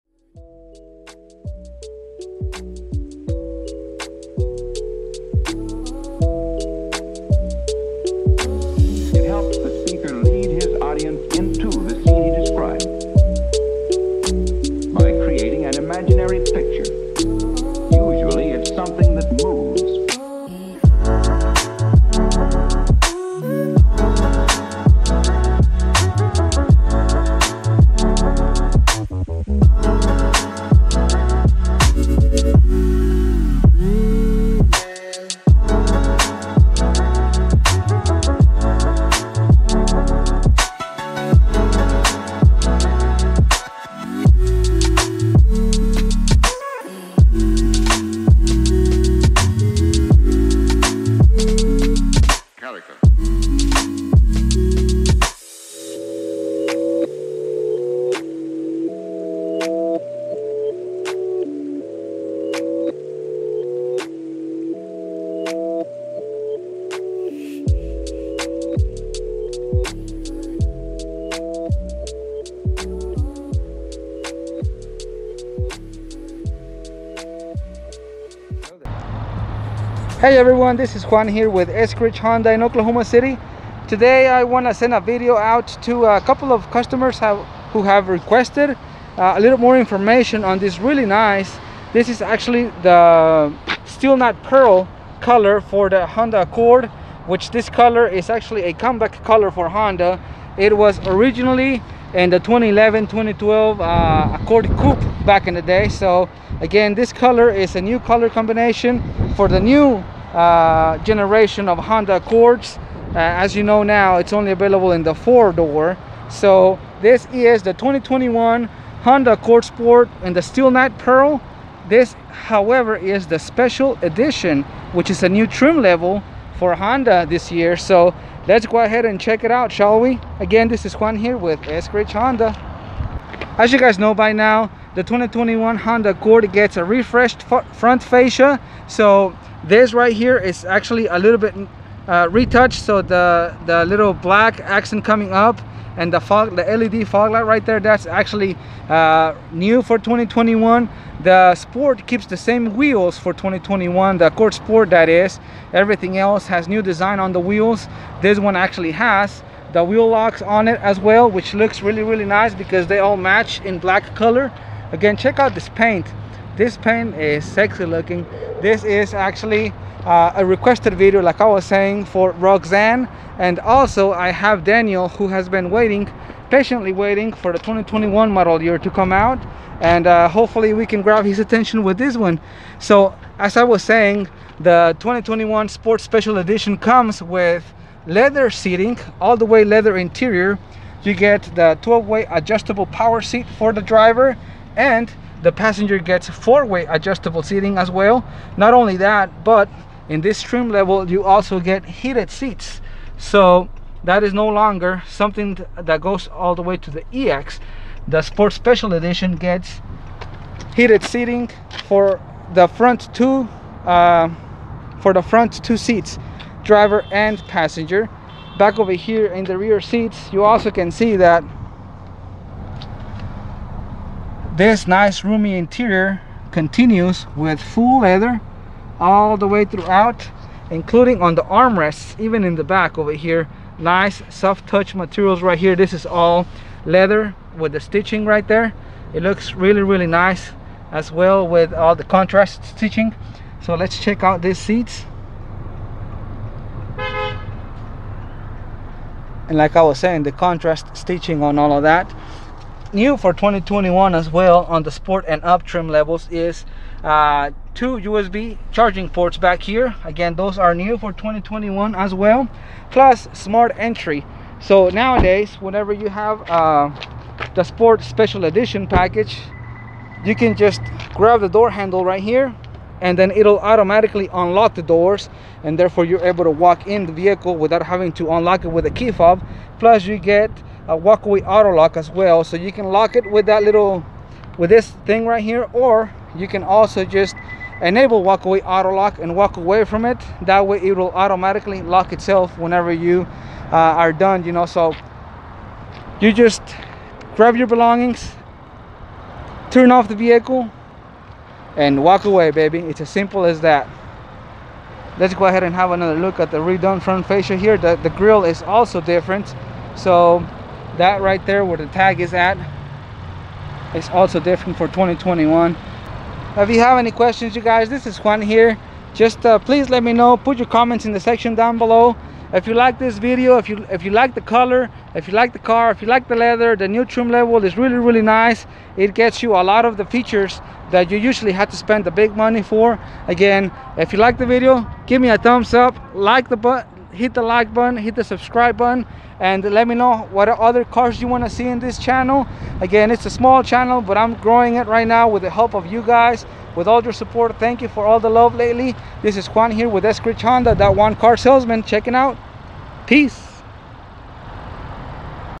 2021 Accord Sport Special Edition
Hi every one! This video goes out Roxanne and also Daniel who have been patiently waiting for me to get my hands on a new 2021 Still Night Pearl Accord Sport Special Edition.

Thanks to everyone showing up and supporting the content I put up.

Call the store and ask for Juan: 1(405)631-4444